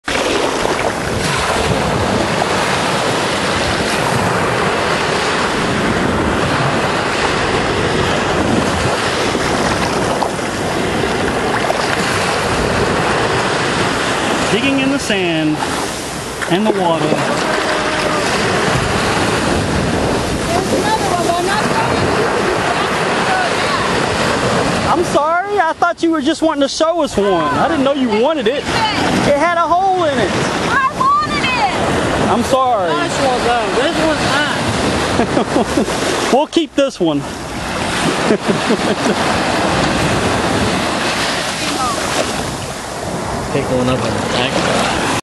Throwing shells back
Tossing shells back into the gulf. or did we?? Having a little fun while searching for shells.